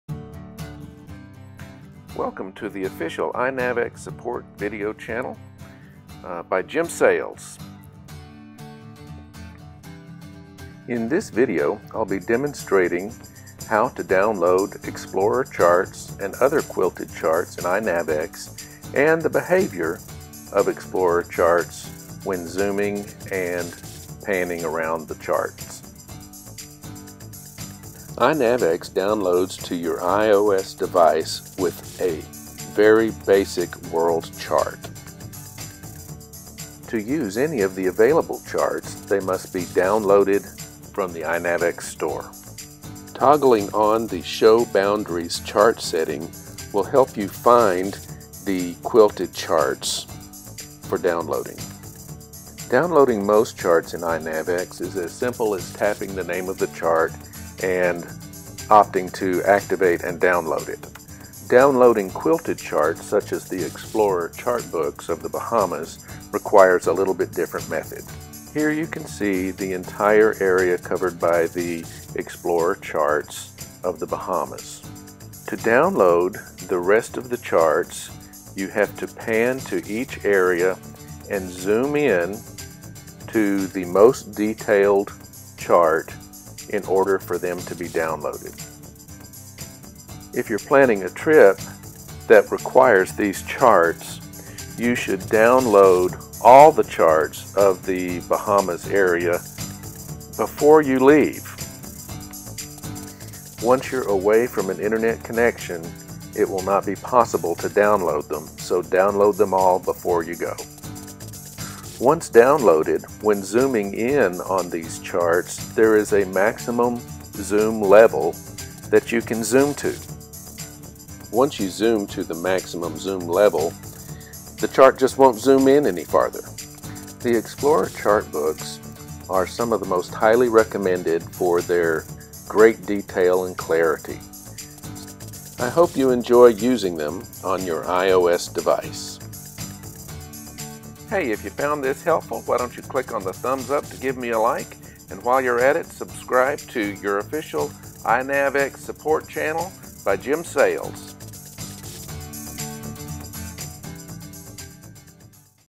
Explorer Charts Bahamas - iOS
This video demonstrates and explains how to download Explorer Charts for the Bahamas on your iOS device.